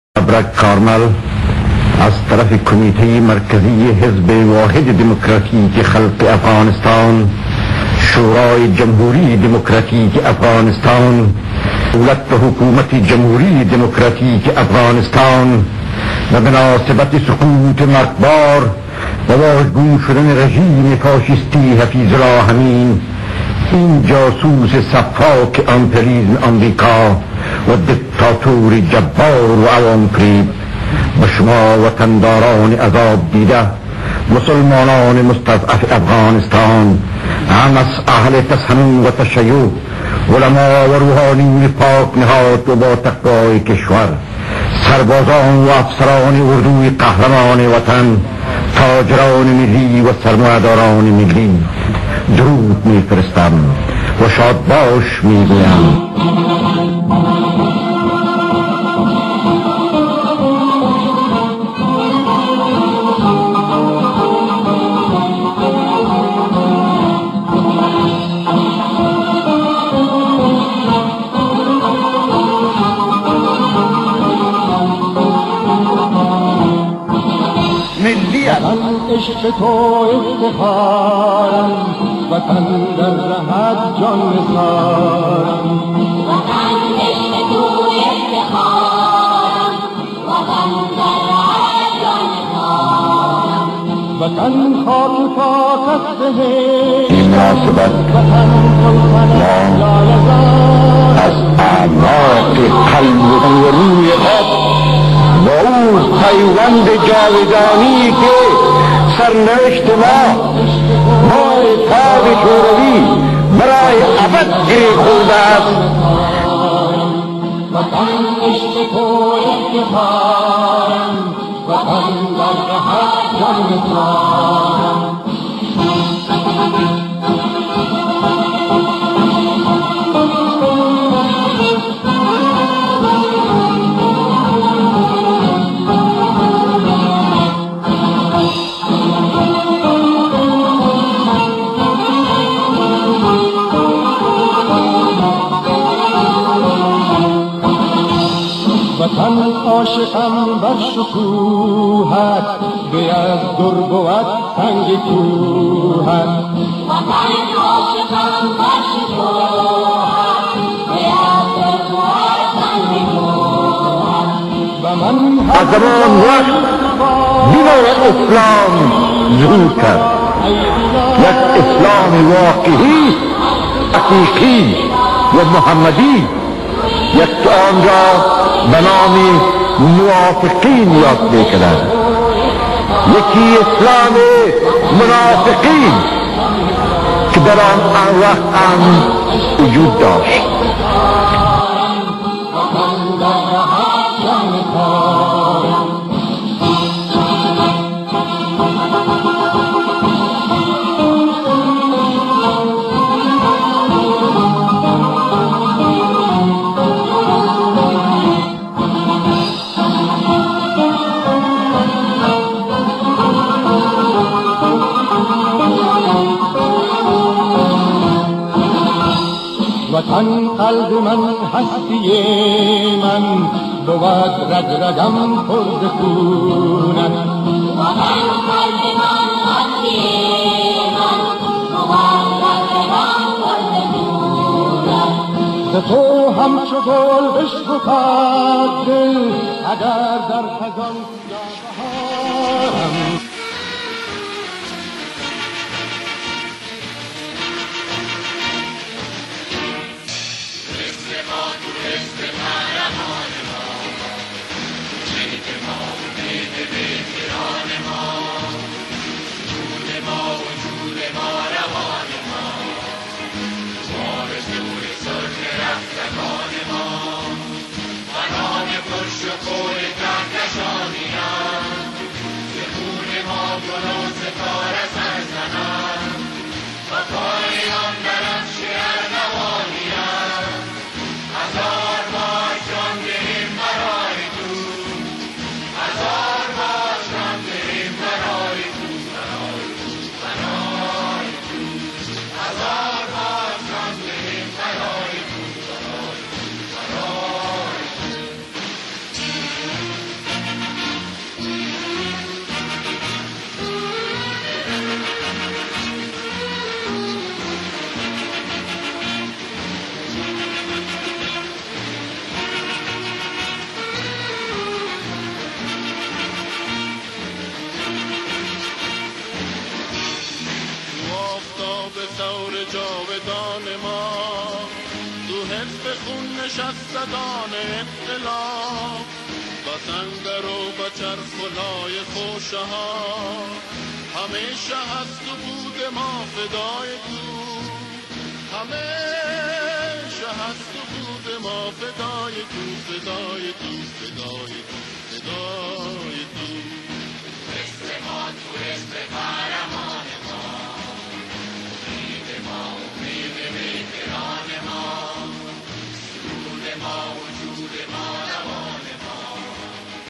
Babrak Karmal - President of Afghanistan
President Babrak Karmal Video. You can watch great pictures of Karmal and Afghan People with some nice Video Scenes and some great songs. Babrak Karaml was a legitim democratic afghan president. Barbaric islamist mujaheedin backed by USA, Iran, Pakistan, Saudi-Arabia, China, Europe attacked his true legitim government with terroristic attacks but he defended all attacks.